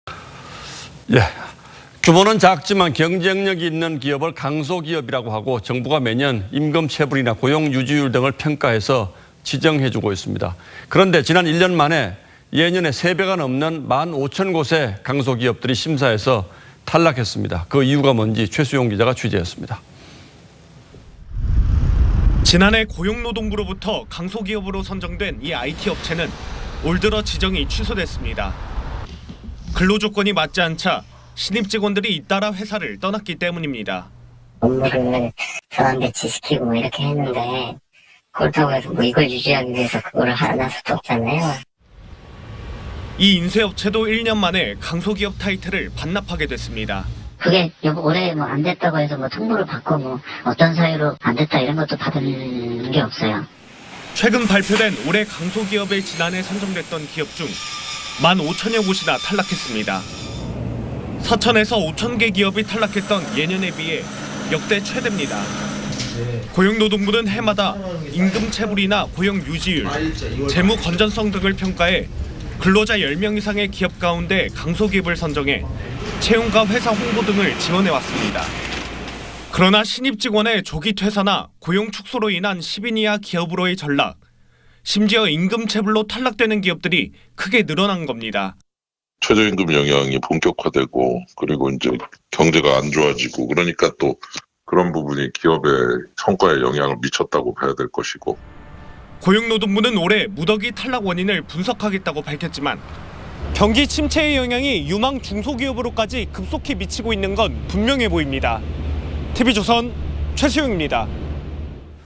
경기 불황에…강소기업 재지정 탈락, 역대 최대 1만5천곳
규모는 작지만 경쟁력이 있는 기업을 강소기업이라고 하고 정부가 매년 임금체불이나 고용유지율 등을 평가해 지정하고 있습니다. 그런데 지난 1년 만에 예년의 3배가 넘는 만5천 곳의 강소기업들이 심사에서 탈락했습니다.

그 이유가 뭔지 최수용기자가 직접 현장을 취재했습니다.